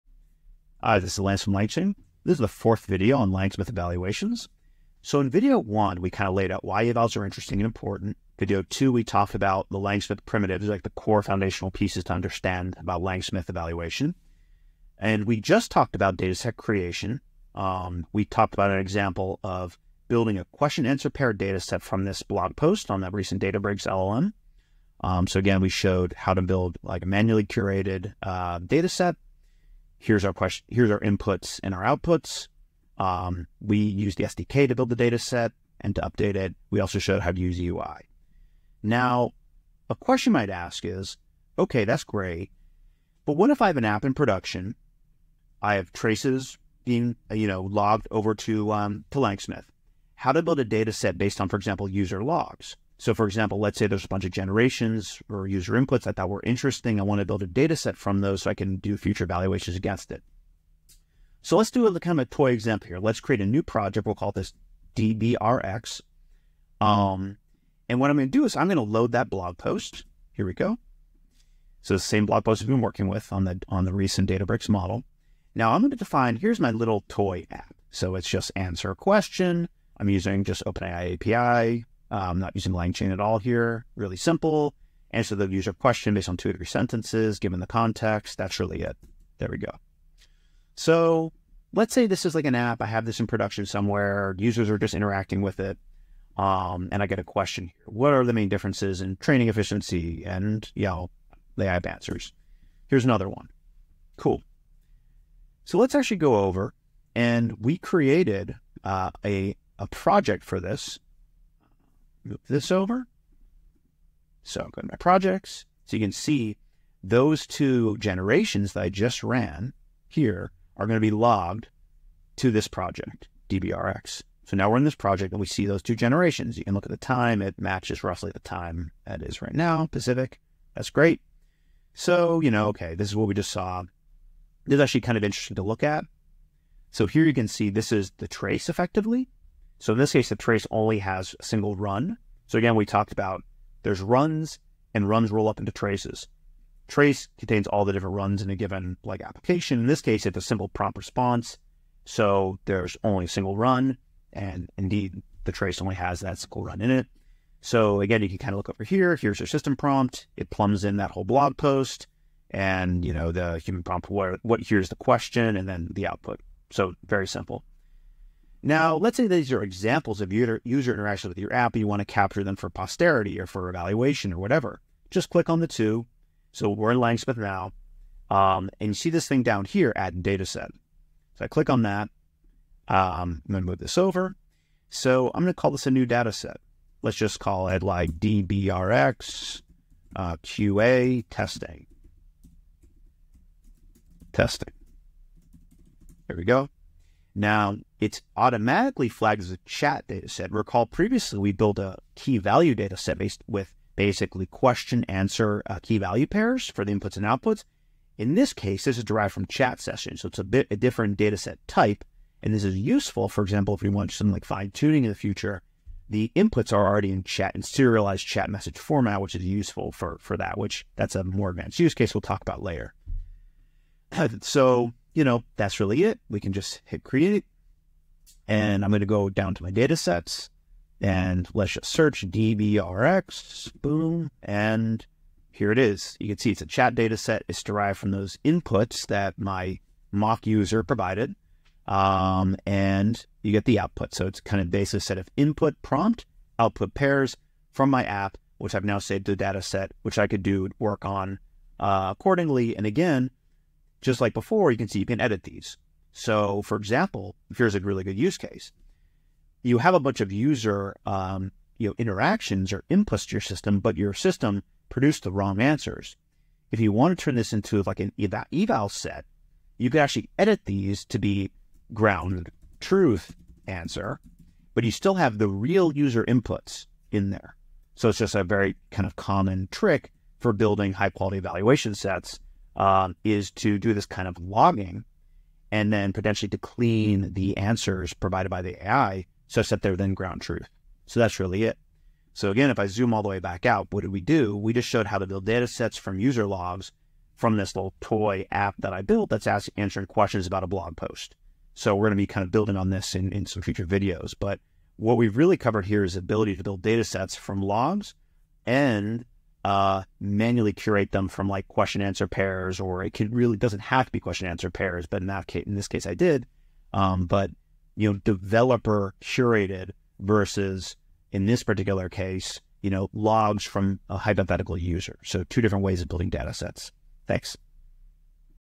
Datasets From Traces | LangSmith Evaluations - Part 4
With the rapid pace of AI, developers are often faced with a paradox of choice: how to choose the right prompt, how to trade-off LLM quality vs cost? Evaluations can accelerate development with  structured process for making these decisions. But, we've heard that it is challenging to get started. So, we are launching a series of short videos focused on explaining how to perform evaluations using LangSmith.

This video introduces how to create datasets directly from logs (e.g., user interactions with your app that are captured in LangSmith).